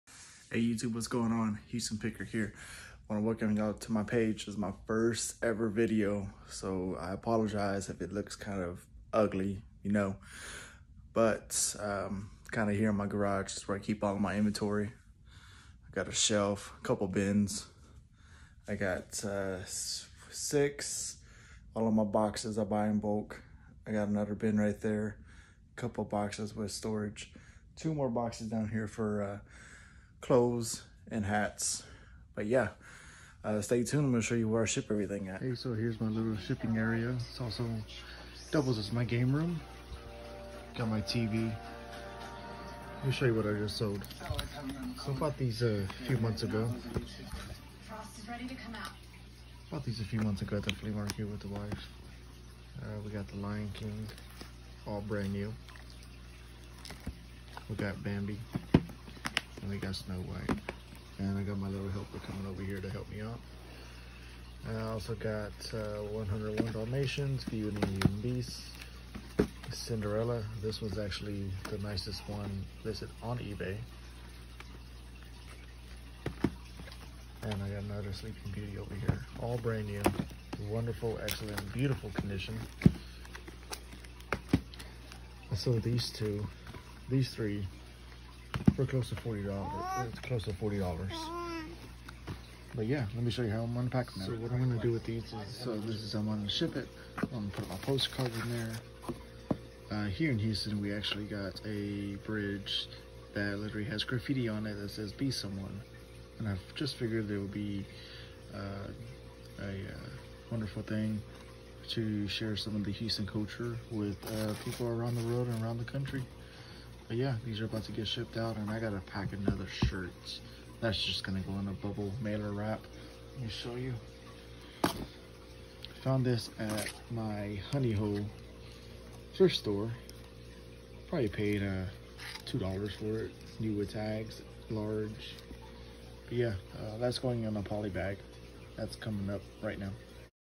I sold these new DISNEY VHS FOR “Diamond” prices- my first YOUTUBE video!
Welcome to my new YouTube page I promise with time the videos will get better!